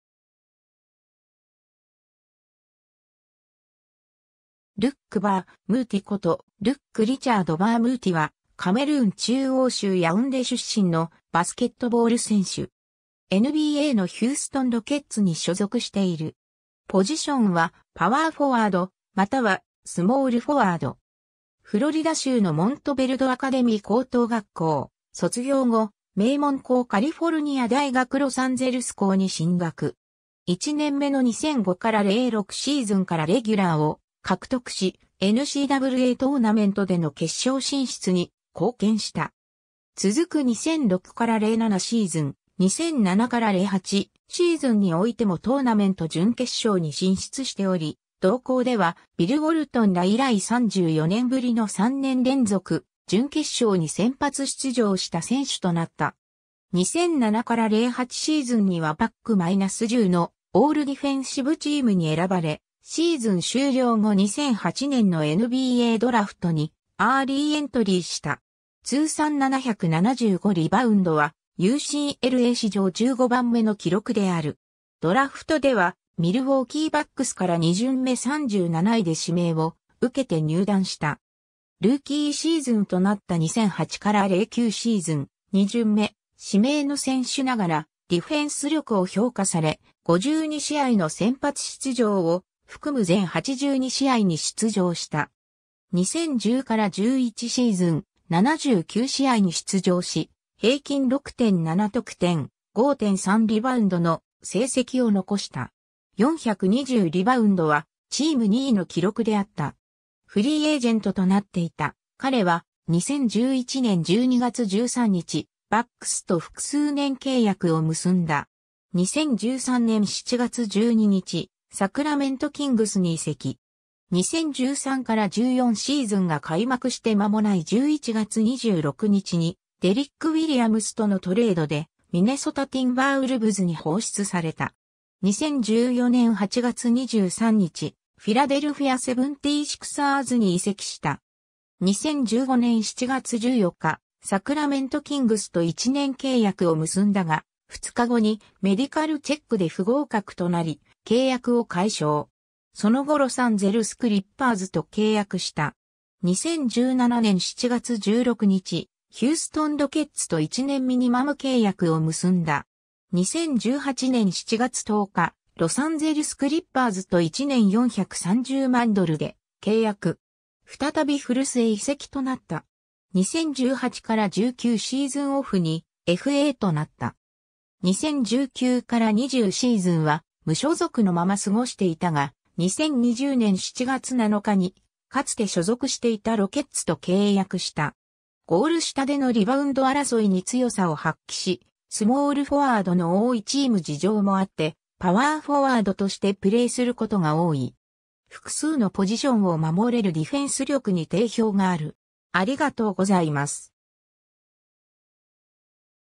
ルック・バ・ア・ムーティ
ルック・バ・ア・ムーティ（Luc Mbah a Moute 発音 BAH-ah-MOO-tay ）ことルック・リチャード・バ・ア・ムーティ（Luc Richard Mbah a Moute, 1986年9月9日 - ）は、カメルーン中央州ヤウンデ出身のバスケットボール選手。
NBAのヒューストン・ロケッツに所属している。
ポジションはパワーフォワードまたはスモールフォワード。
 フロリダ州のモントベルドアカデミー高等学校卒業後、名門校カリフォルニア大学ロサンゼルス校（UCLA）に進学。
1年目の2005-06シーズンからレギュラーを獲得し、NCAAトーナメントでの決勝進出に貢献した。
 続く2006-07シーズン、2007-08シーズンにおいてもトーナメント準決勝に進出しており、同校ではビル・ウォルトンら以来34年ぶりの3年連続準決勝に先発出場した選手となった。
 2007-08シーズンにはPac-10のオールディフェンシブチームに選ばれ、シーズン終了後2008年のNBAドラフトにアーリーエントリーした。
通算775リバウンドはUCLA史上15番目の記録である。
 ドラフトではミルウォーキー・バックスから2巡目37位で指名を受けて入団した。
 ルーキーシーズンとなった2008-09シーズン、2巡目指名の選手ながらディフェンス力を評価され、52試合の先発出場を含む全82試合に出場した。
 2010-11シーズン、79試合に出場し平均6.7得点、5.3リバウンドの成績を残した。
420リバウンドはチーム2位の記録であった。
フリーエージェントとなっていた彼は、2011年12月13日、バックスと複数年契約を結んだ。
 2013年7月12日、サクラメント・キングスに移籍。
 2013-14シーズンが開幕して間もない11月26日に、デリック・ウィリアムスとのトレードでミネソタ・ティンバーウルブズに放出された。
 2014年8月23日、フィラデルフィア・76ersに移籍した。
 2015年7月14日、サクラメント・キングスと1年契約を結んだが、2日後にメディカルチェックで不合格となり、契約を解消。
その後ロサンゼルス・クリッパーズと契約した。
 2017年7月16日、ヒューストン・ロケッツと1年ミニマム契約を結んだ。
 2018年7月10日、ロサンゼルス・クリッパーズと1年430万ドルで契約。
再び古巣へ移籍となった。
2018-19シーズンオフにFAとなった。
 2019-20シーズンは無所属のまま過ごしていたが、2020年7月7日にかつて所属していたロケッツと契約した。
 ・ゴール下でのリバウンド争いに強さを発揮し、スモールフォワードの多いチーム事情もあってパワーフォワードとしてプレーすることが多い。
 ・複数のポジションを守れるディフェンス力に定評がある。